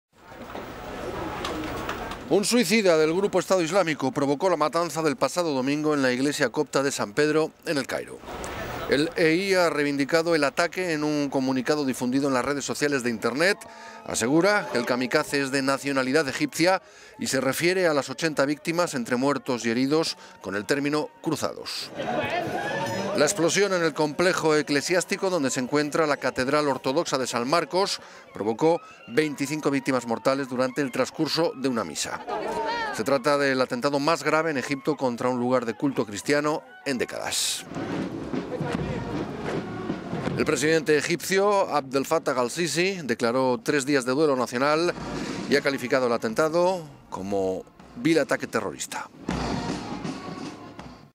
El grupo Estado Islámico reivindica el atentado contra la iglesia copta de San Pedro en el Cairo
Un suicida del grupo Estado Islámico provocó la matanza del pasado domingo en la iglesia copta de San Pedro en el Cairo.

El EI ha reinvindicado el ataque en un comunicado difundido en las redes sociales de Internet, asegura que el kamikaze es de nacionalidad egipcia y se refiere a las 80 víctimas entre muertos y heridos con el término "cruzados".

La explosión en el complejo eclesiástico donde se encuentra la catedral ortodoxa de San Marcos, provocó 25 víctimas mortales durante el transcurso d…